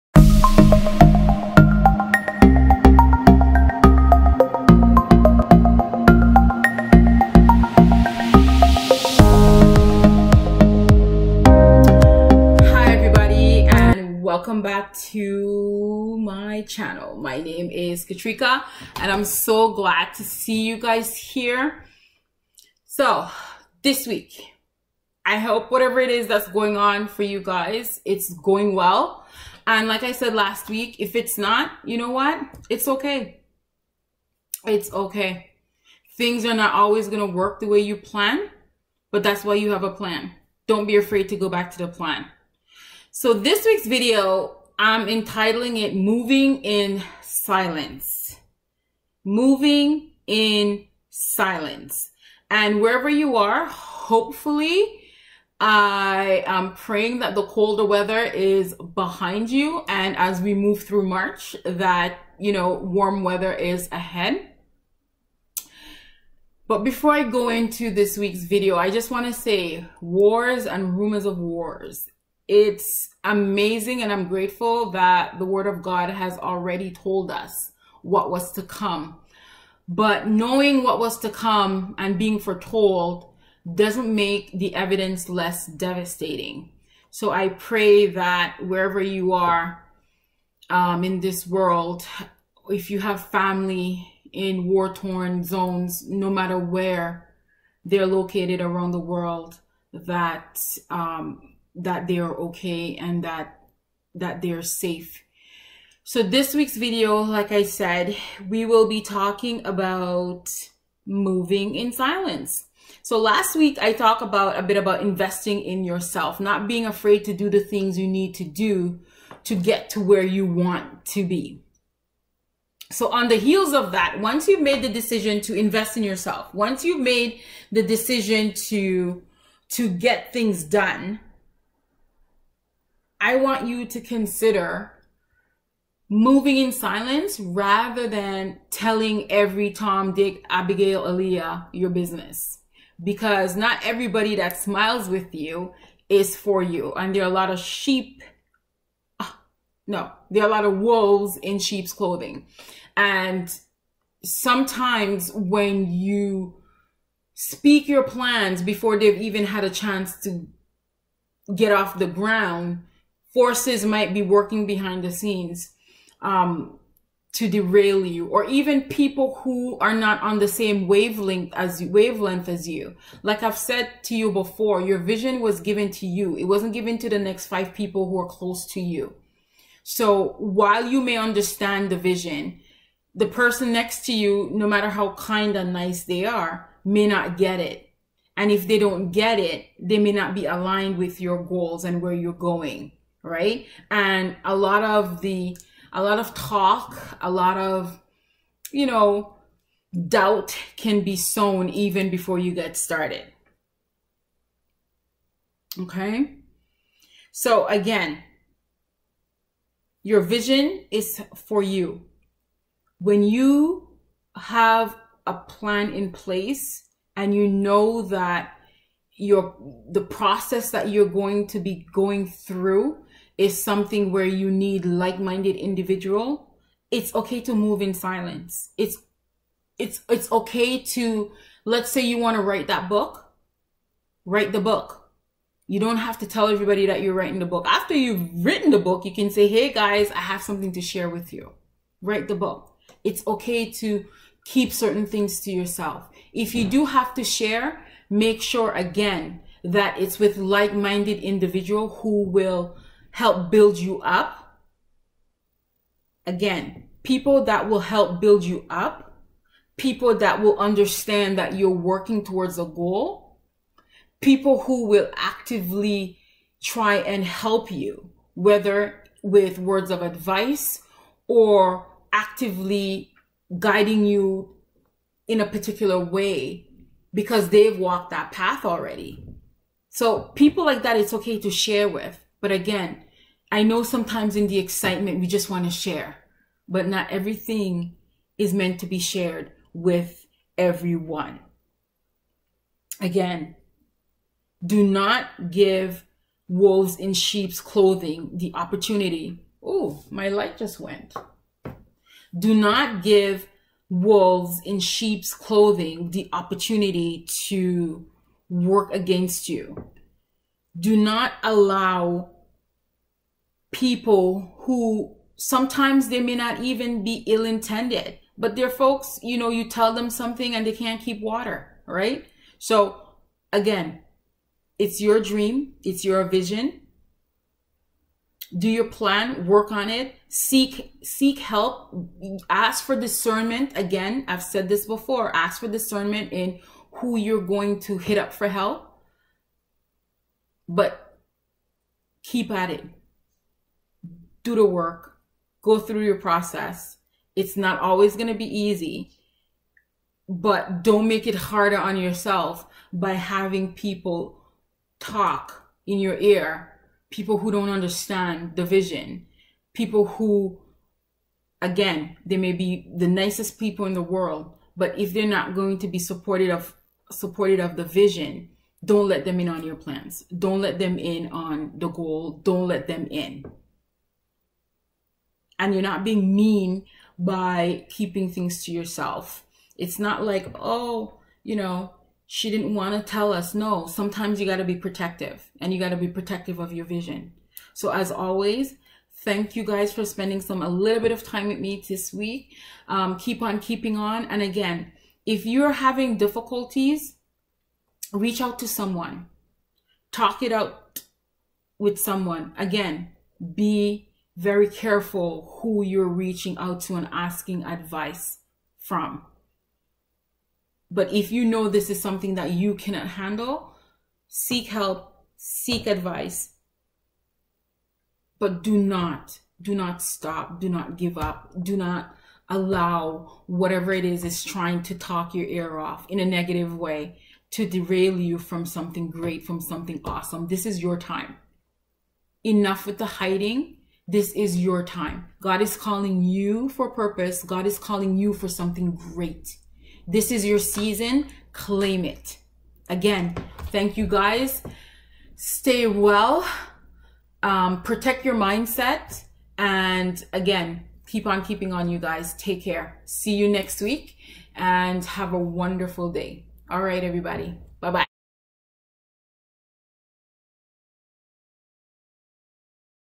Level up with WU Wednesday
Moving in silence to protect your vision.  I'm encouraging you to seek wise counsel which will allow you to advance your goals without jeopardizing them with people who low key wants to see you fail.  As always, take care and keep on keeping on.